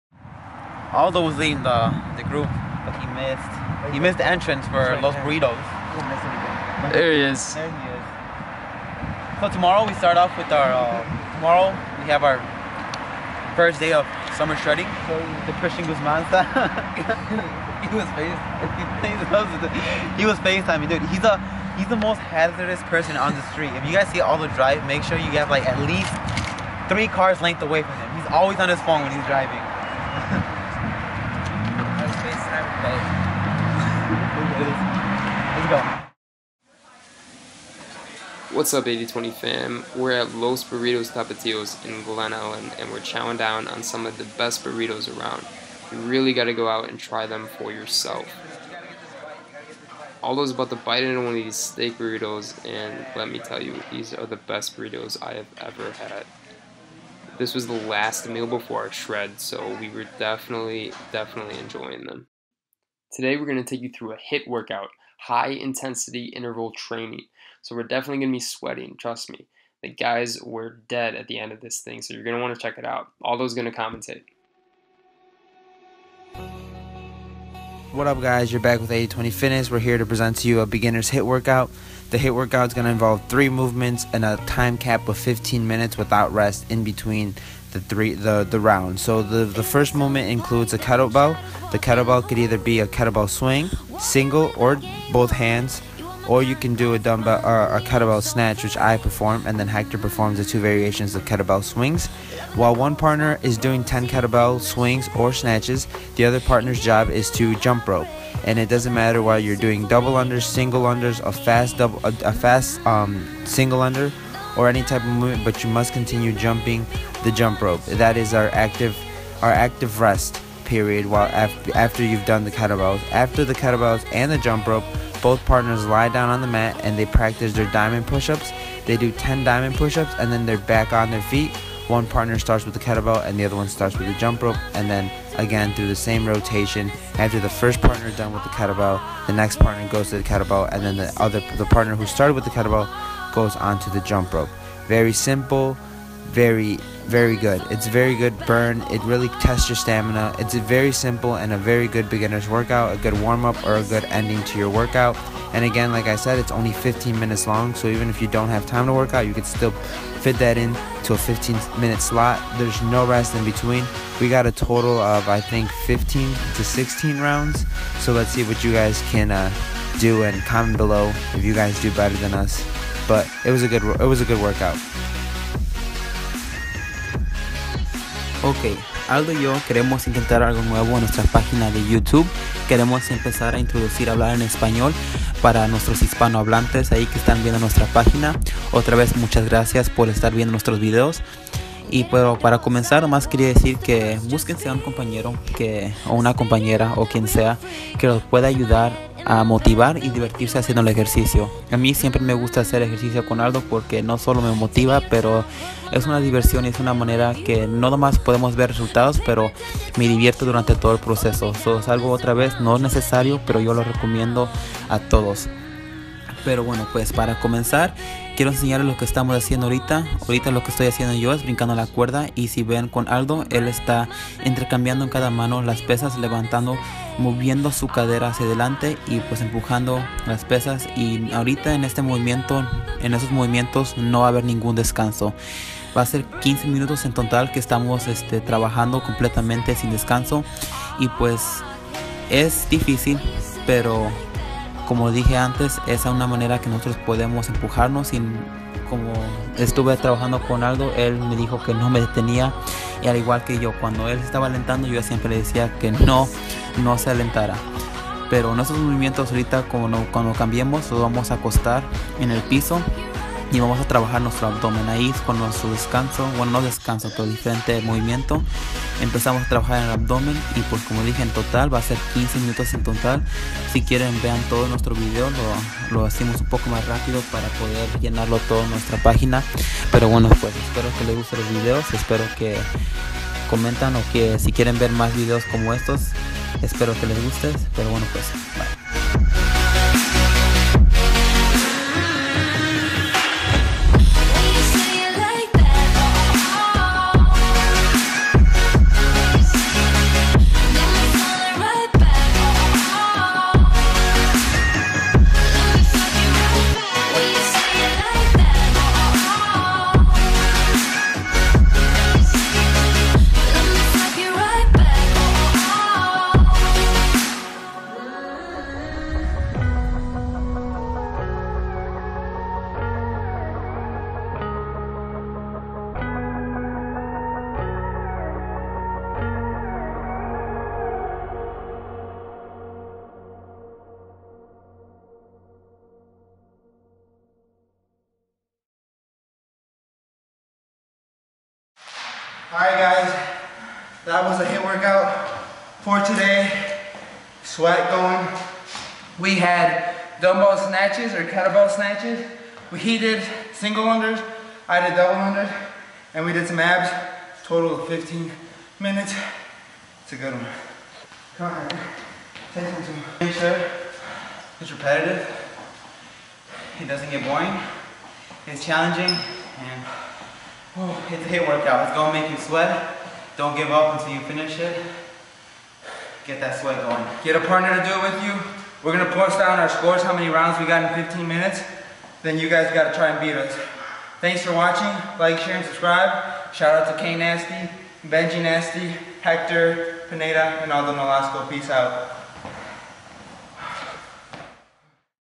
Episode 9 - Beginner's HIIT Workout
Since we have started our shredd, we obviously need to include more cardio in our daily workouts but cardio can get boring! So here we decided to demonstrate a simple HIIT workout that will get your heart going and the fat burning! It is very simple and really fun, try it with a swolemate! We got through 15 rounds! Comment your score when you're done!

Aldo: aldo910
Héctor: hector565
Kyle: knasty2396